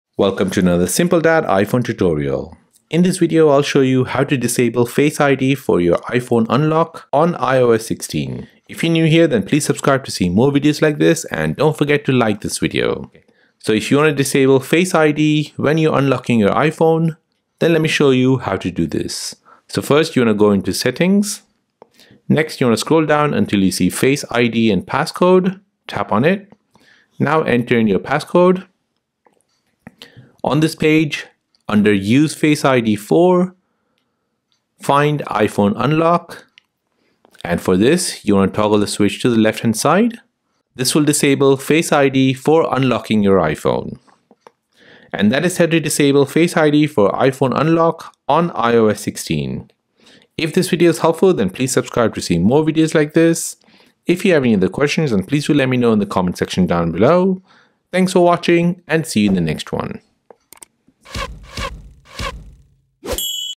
How to Disable Face Id for IPhone Unlock on iOS 16 (2023)
00:00 Intro
00:10 Like or no Like
00:20 How to Disable Face Id for IPhone Unlock on iOS 16